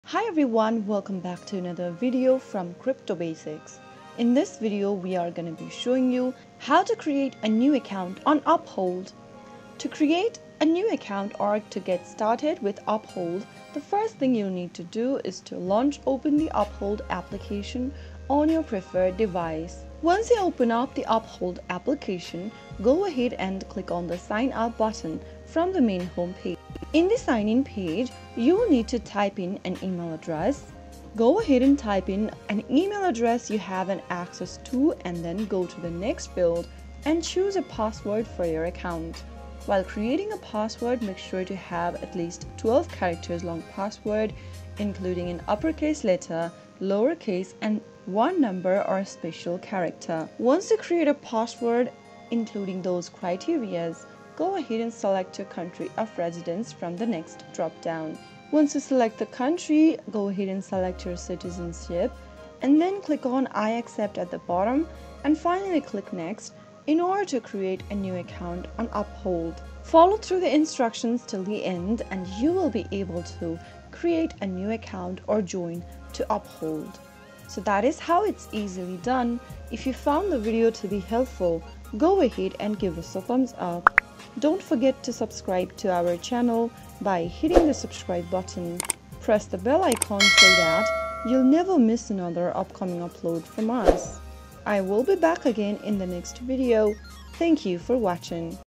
How to Create/Open New Uphold Account 2024?
Welcome to our comprehensive tutorial on how to create Uphold account! Uphold is a versatile digital wallet and trading platform that allows you to manage various assets, including cryptocurrencies, fiat currencies, and precious metals.
In this video, we'll walk you through the process of setting up your Uphold account from start to finish.
By creating an Uphold account, you can easily buy, sell, and manage a wide range of assets, all in one place. Whether you're new to digital wallets or an experienced trader, this tutorial will help you get started quickly and securely.